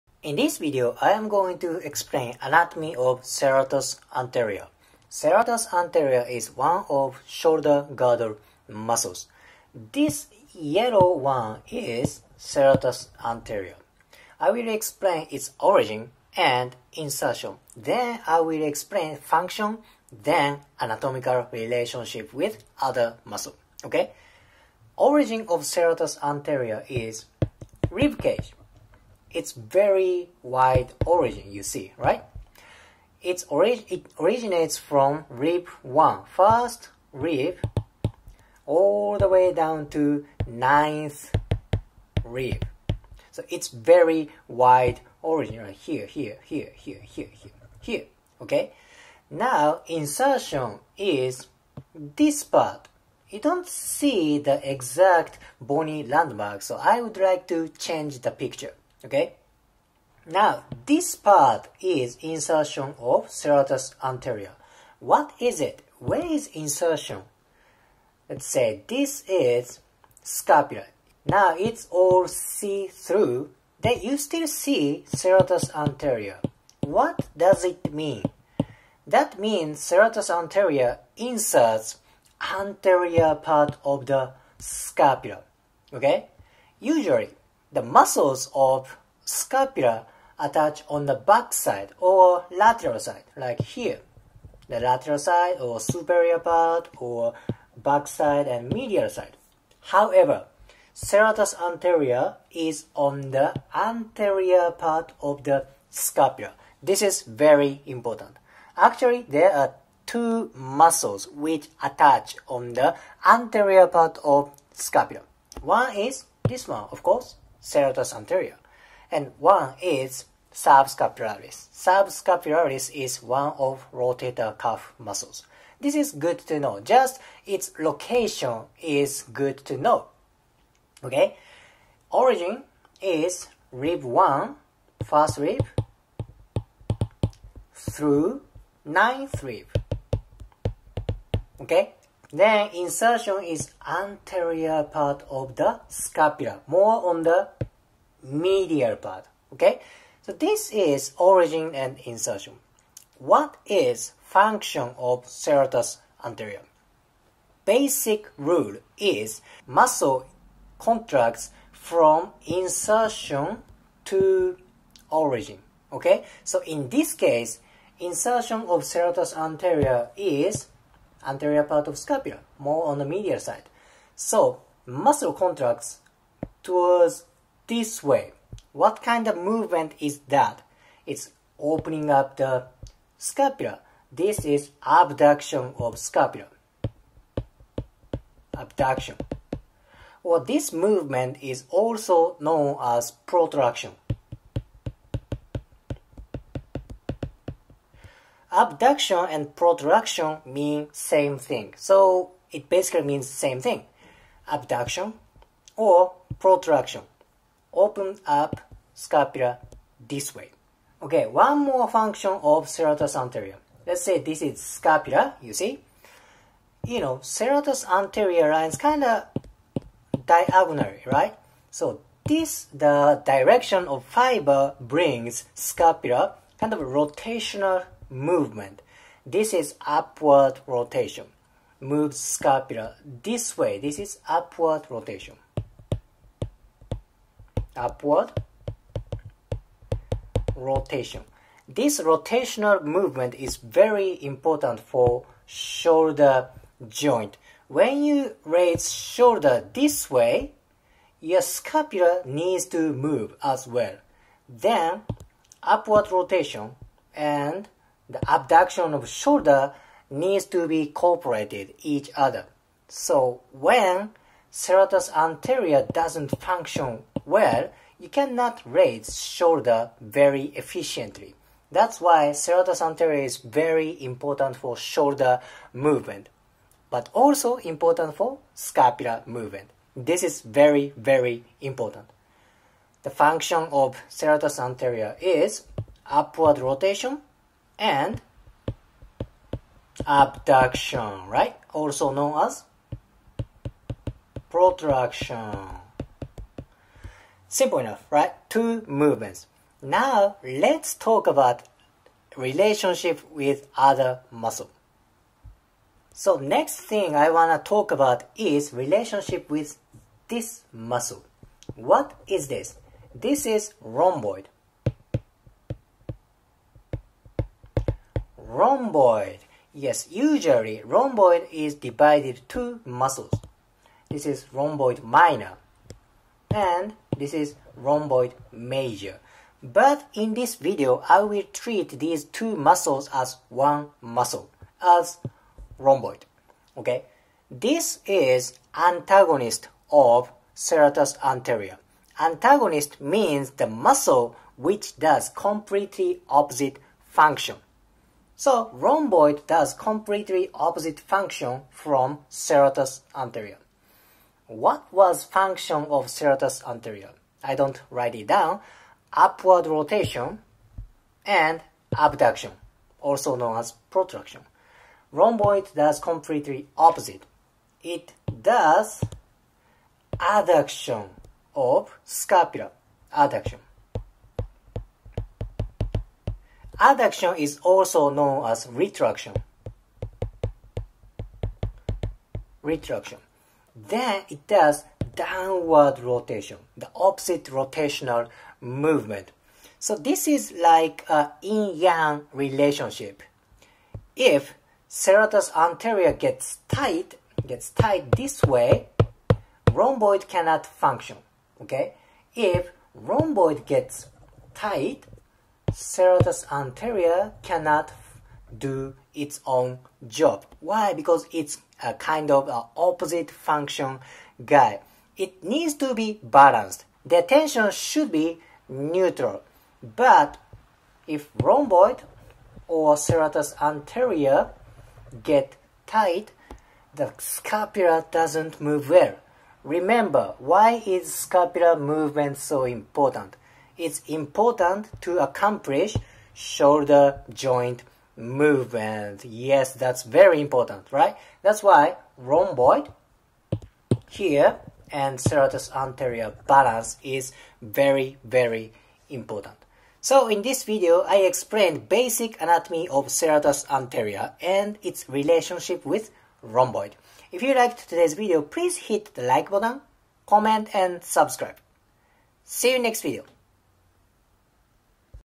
Anatomy and function of serratus anterior (English)
In this video, I explain anatomy of serratus anterior.